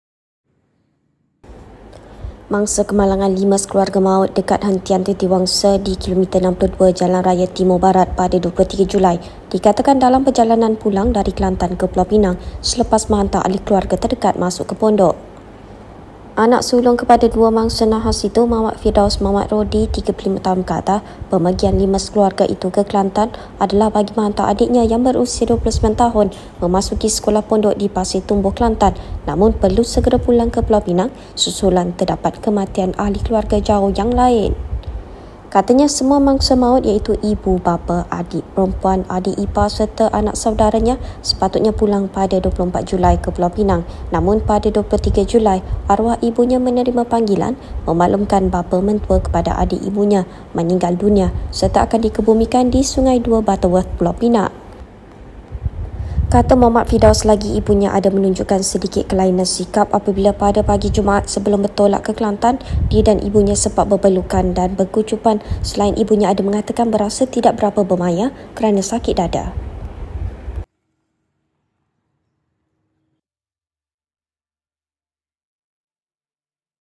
Hantar keluarga masuk pondok
Mangsa kemalangan lima sekeluarga maut dekat Hentian Titiwangsa, di KM62, Jalan Raya Timur-Barat, pada 23 Julai dikatakan dalam perjalanan pulang dari Kelantan ke Pulau Pinang selepas menghantar ahli keluarga terdekat masuk ke pondok.